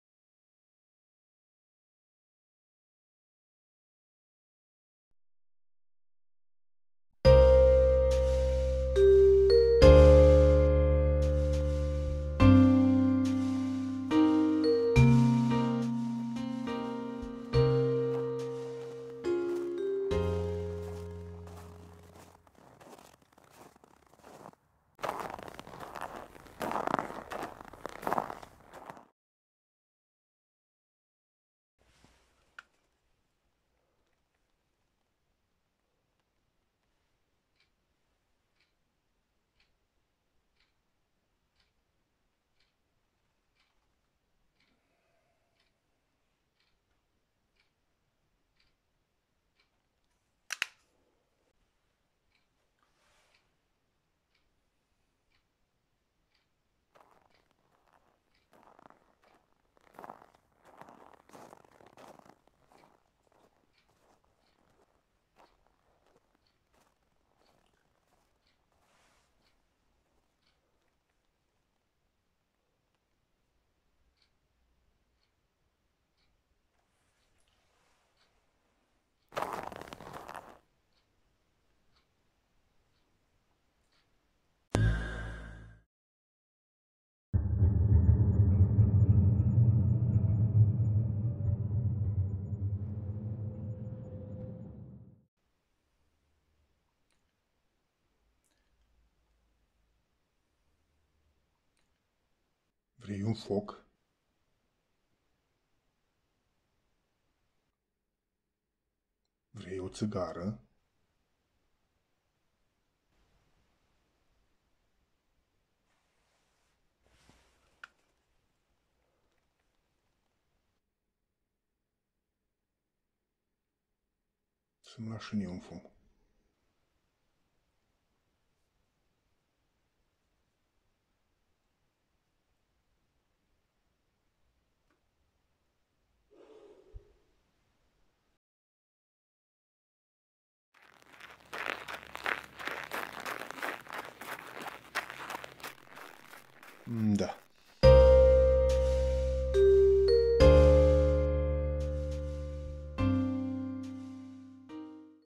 Scurtmetraj Amuzant - FUMATORII - RED BOX BT Media
Neo Noir & Cartoon Style
Genul filmului: Tensionant / Suspans cu sfarsit Amuzant / Comic.
Un foarte scurtmetraj, ce a rezultat in urma unor teste, in care sunt aduse doua tehnici cinematografice. Neo Noir si Cartoon Style.
Teapa fumatorului profesionist :))
Editare: After Effects, Premiere.

În noaptea liniștită de iarnă, fulgii de zăpadă cad ușor asupra unei figuri solitare care se plimbă prin parc. Bărbatul contemplă dacă să savureze ultima sa țigară. Fără să știe, pași liniștiți se apropie, iar un străin misterios apare, aruncând o privire intensă. Speriat, bărbatul oferă un foc, dar străinul refuză. Curios, acesta întreabă despre o țigară, iar bărbatul acceptă cu reticență. În timp ce figura misterioasă aprinde țigara, un val dens de fum îi învăluie. Profitând de ocazie, străinul fuge, lăsând bărbatul fără ultima sa țigară.